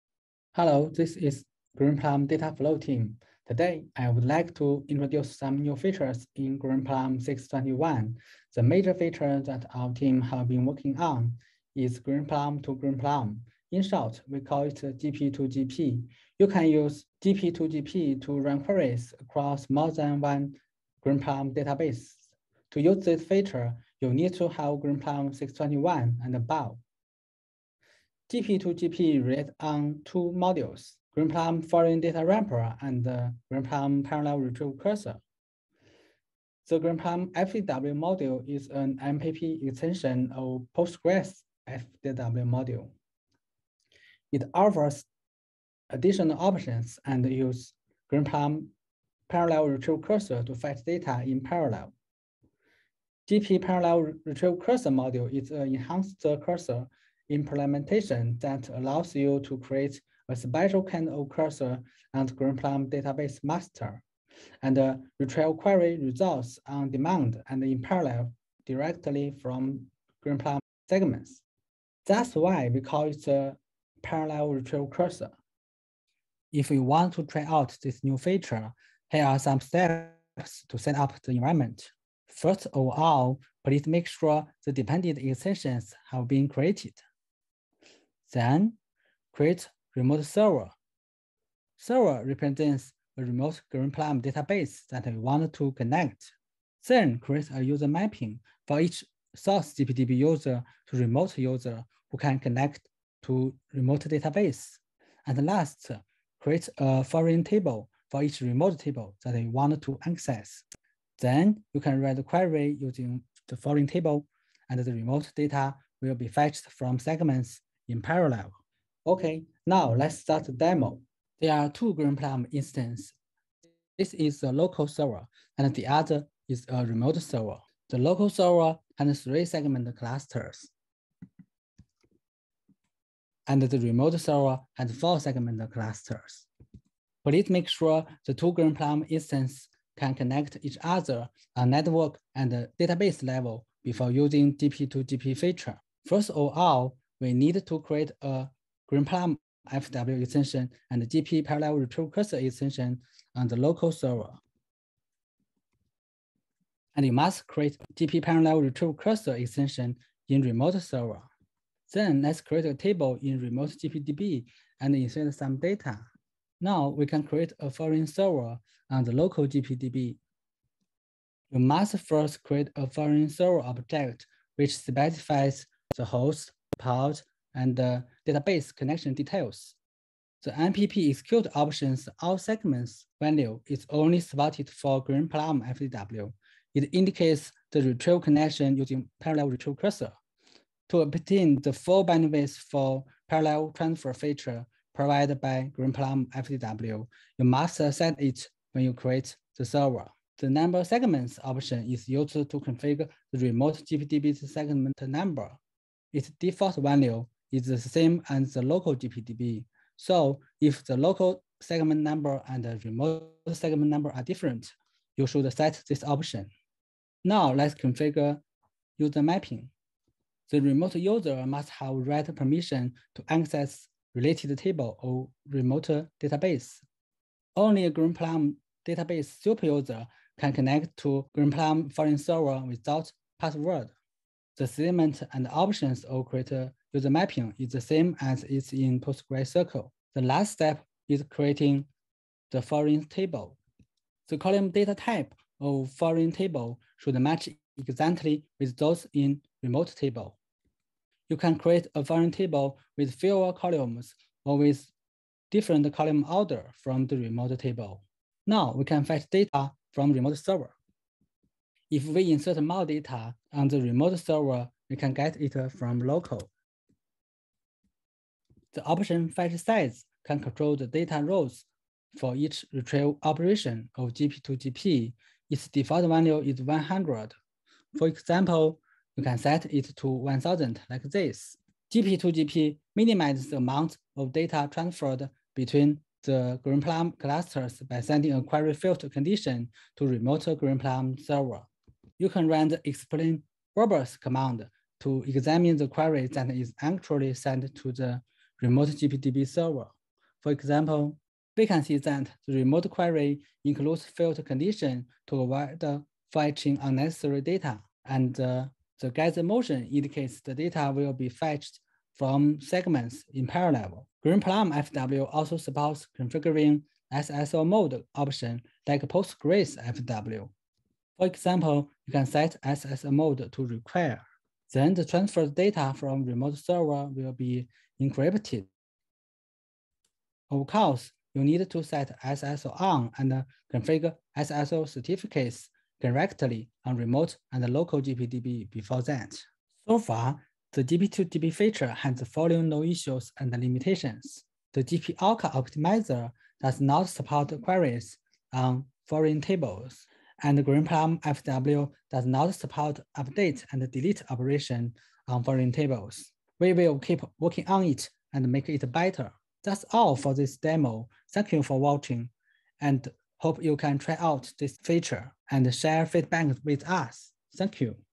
Greenplum 2 Greenplum Federation
Greenplum 2 Greenplum Data Federation allows you to create a grid of clusters by combining multiple clusters together and running queries across the clusters using MPP data distribution to achieve high speed data federation and data sharing while avoiding data duplication.

The GP2GP feature has two parts on the server side is the Parallel Cursor used for ACID compliant and consistent segment to segment data access.  The second part on the client side is the Greenplum Foreign Data Wrapper.